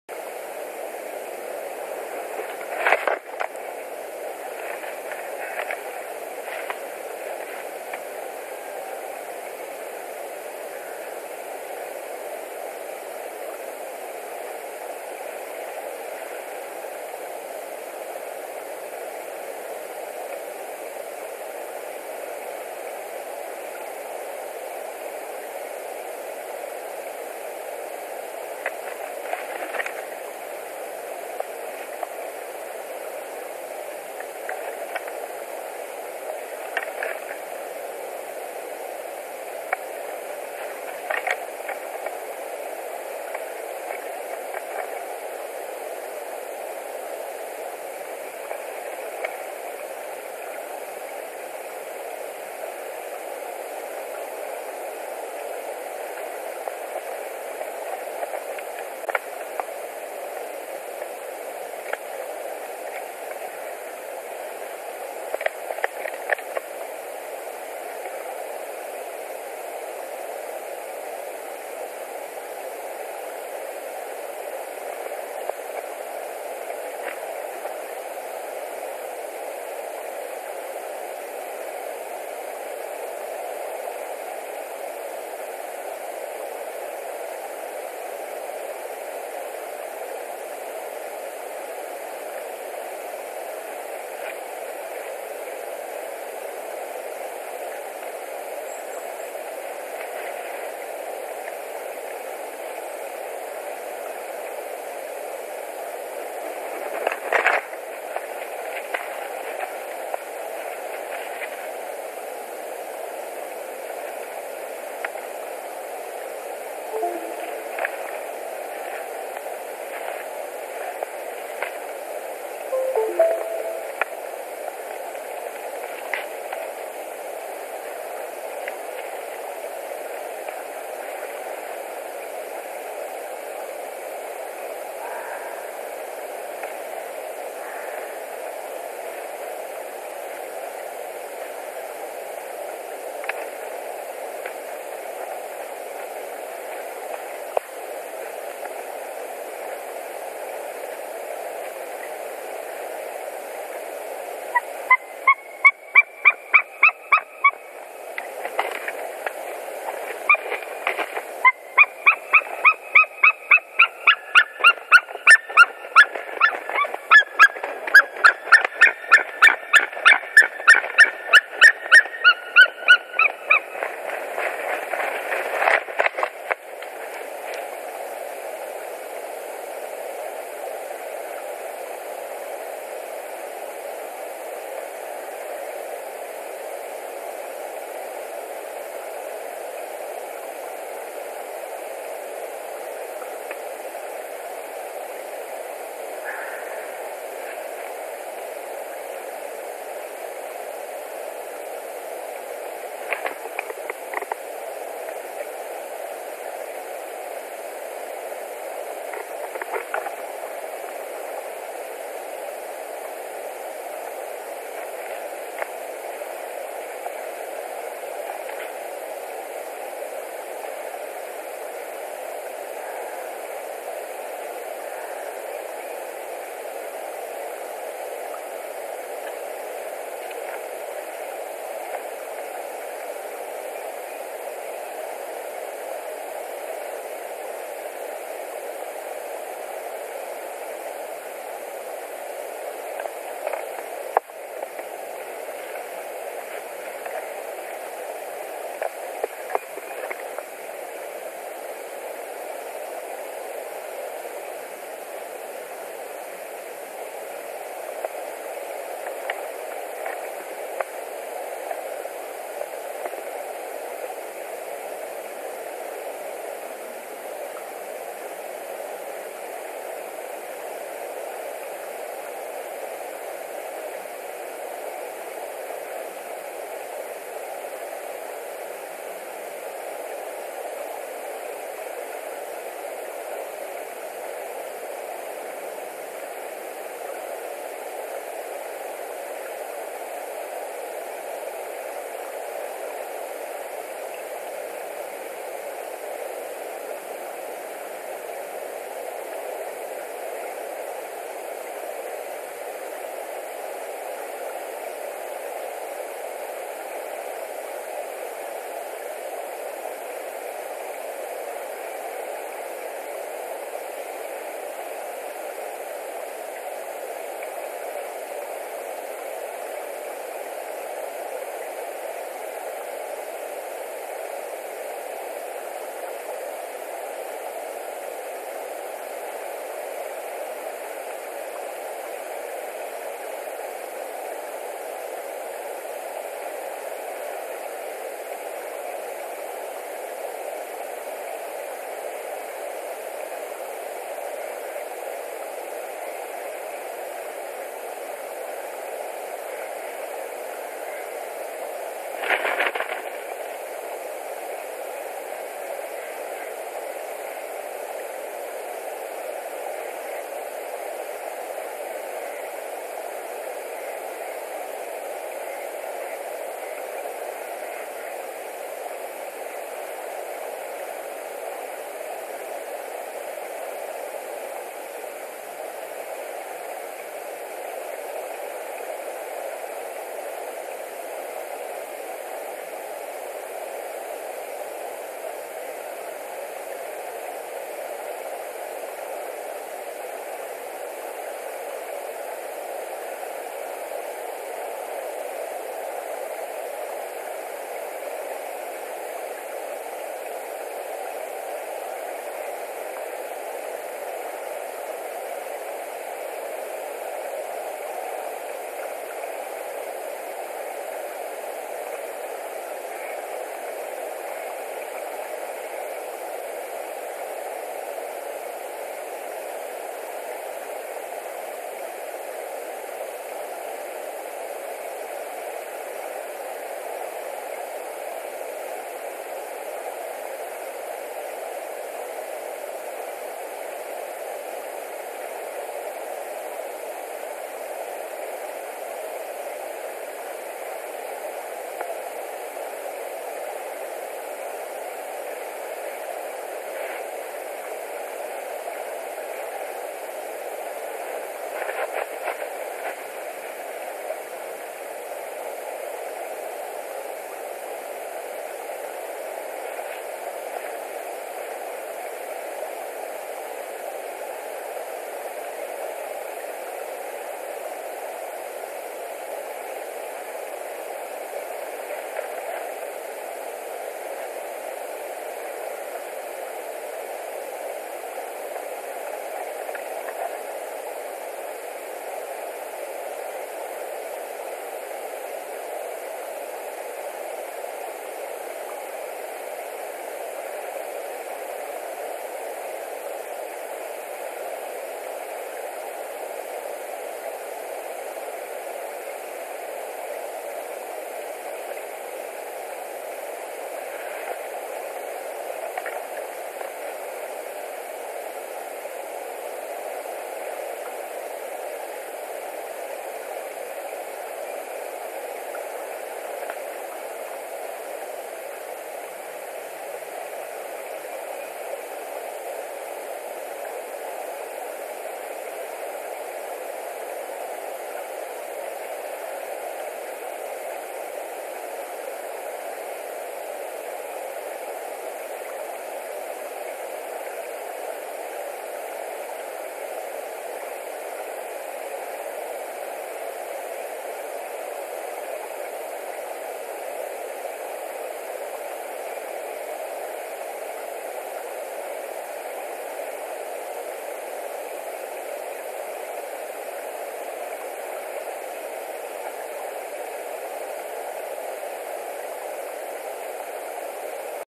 White-tailed Eagles Latvia nest 2019 02 17 Milda, Raims and mating
10:27 Milda with branch
10:28 Raims with branch
10:30 mating